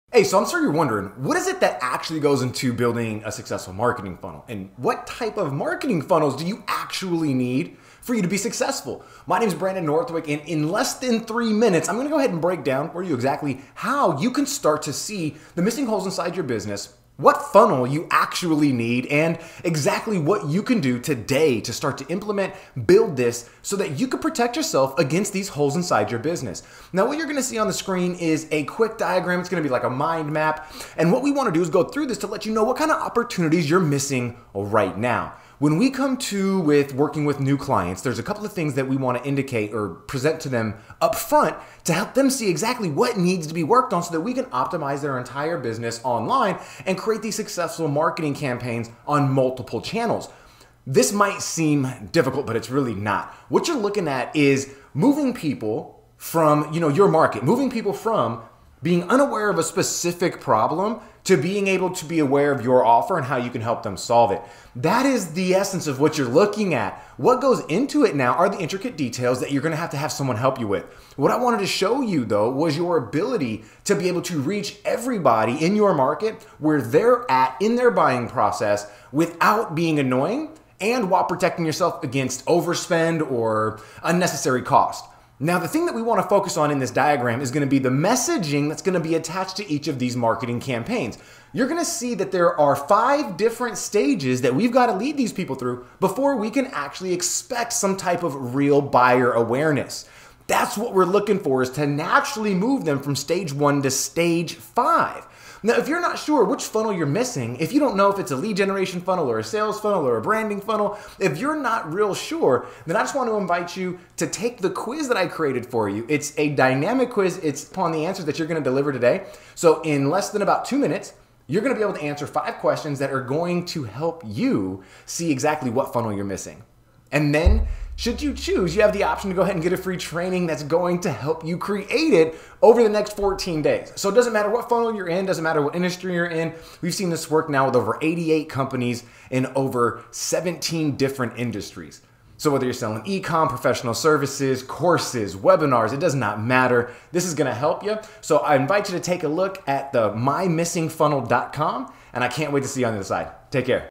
What Is The Best Marketing Funnel For My Business?
Today you'll see which marketing funnel you're missing and the impact your business could have if you added the right one.